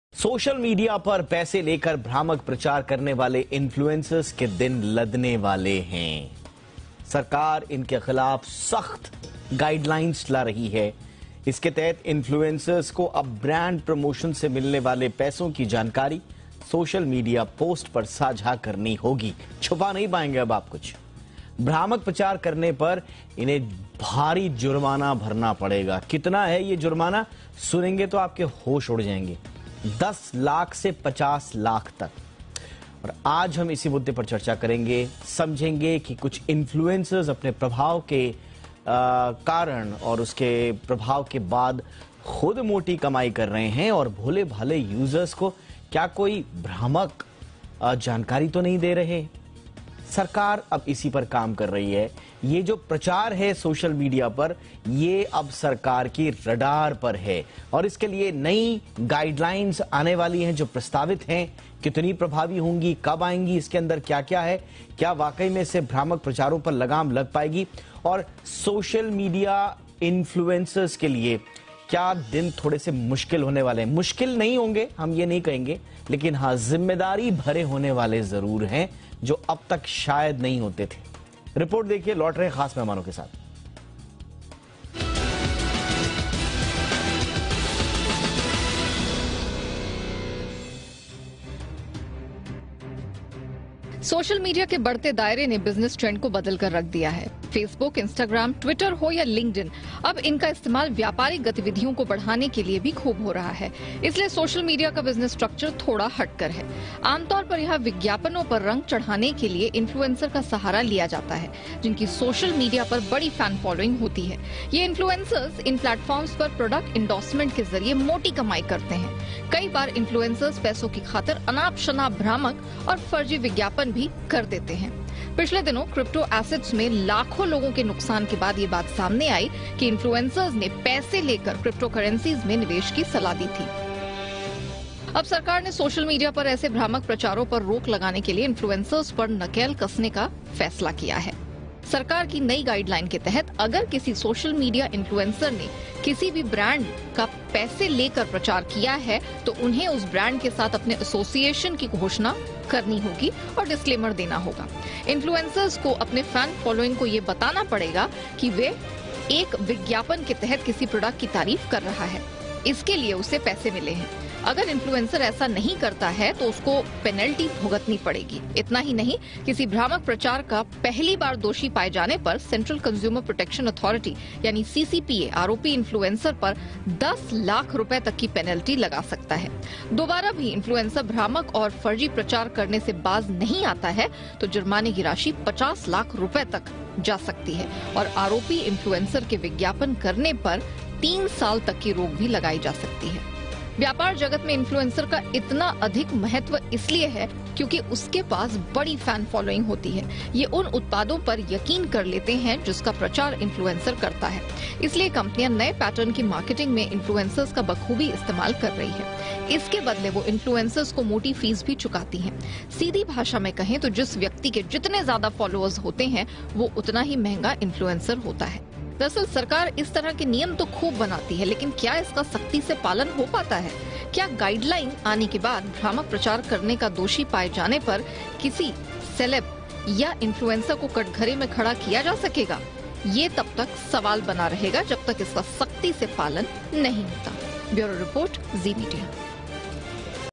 Guidelines for social media influencers on the Product Ads
इन्फ्लुएंसर के लिए नियम सख्त

प्रोडक्ट एड पर देना होगा डिस्क्लेमर

सरकारी रडार पर इन्फ्लुएंसर्स का प्रचार

देखिए ये खास रिपोर्ट